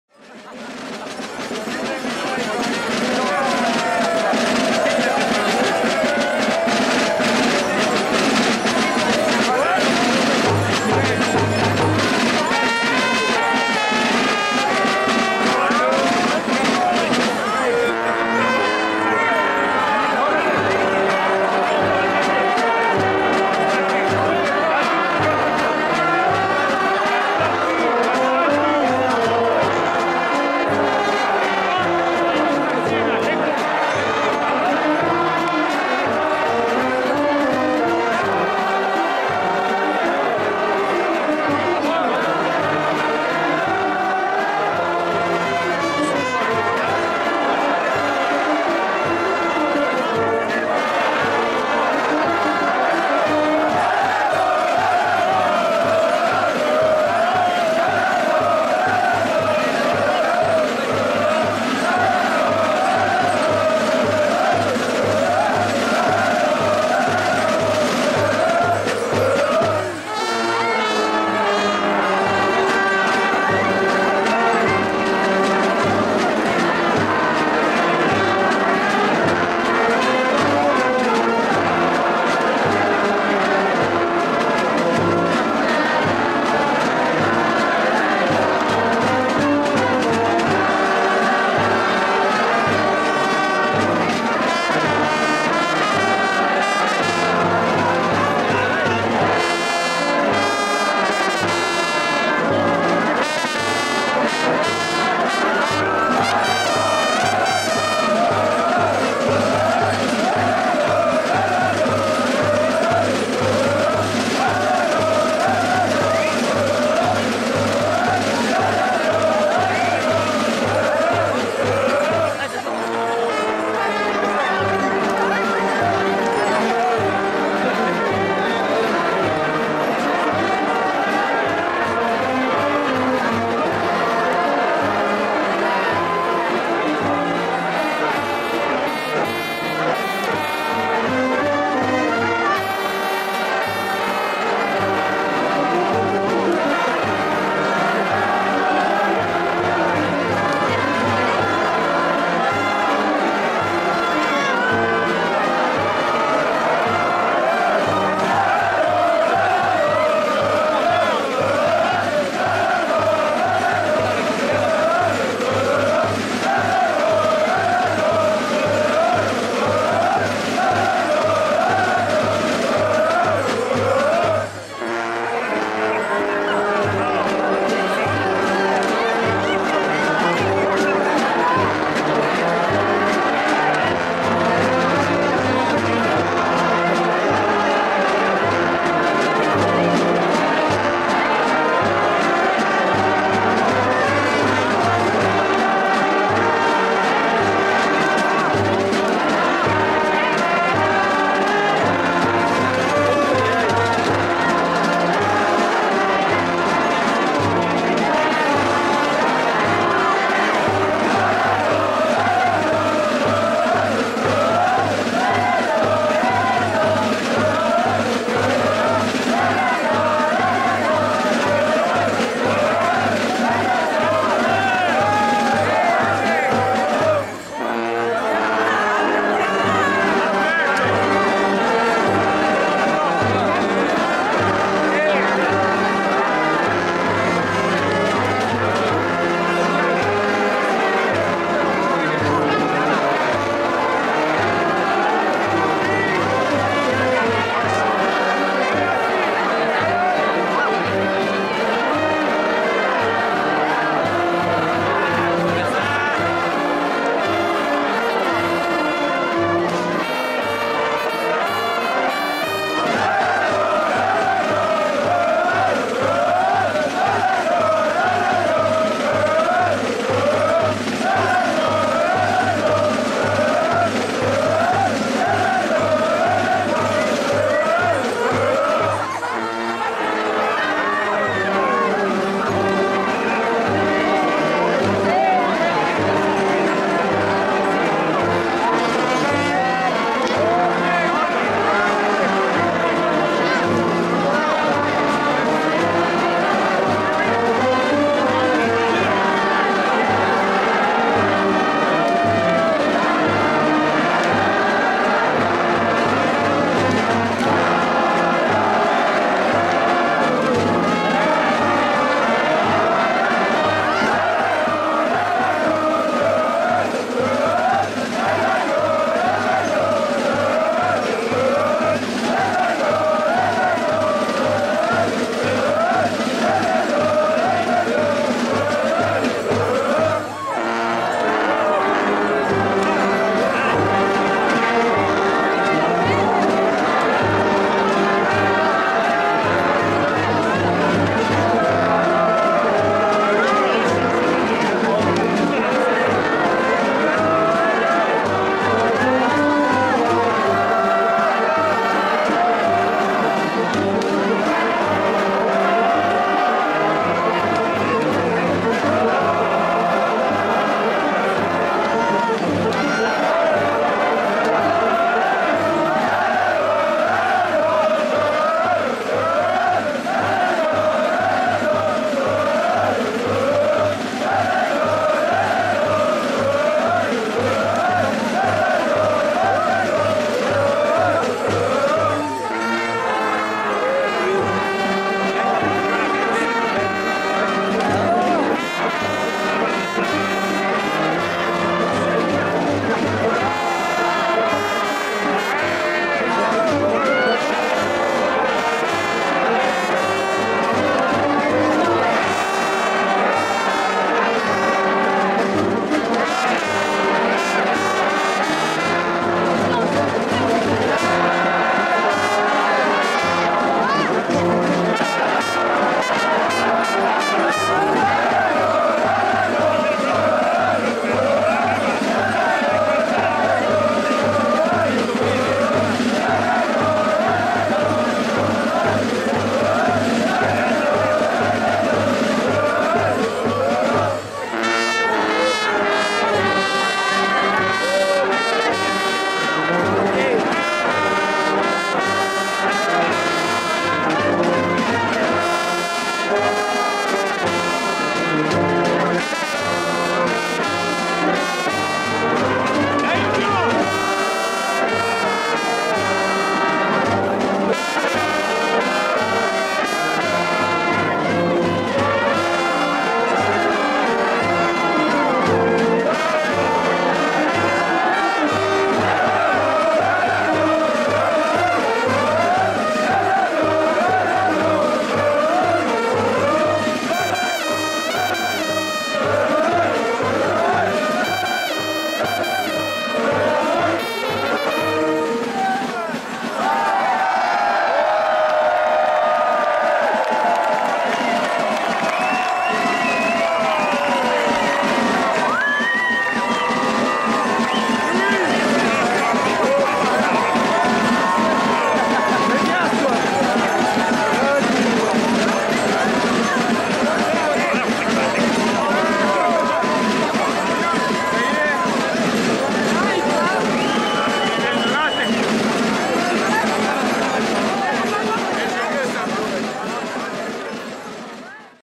Carnaval 2013- G2K- Brûlage des Bosses - Nivelles
Excellente ambiance lors du Brûlage des bosses de la Société des Gilles de l'An 2000 (G2K)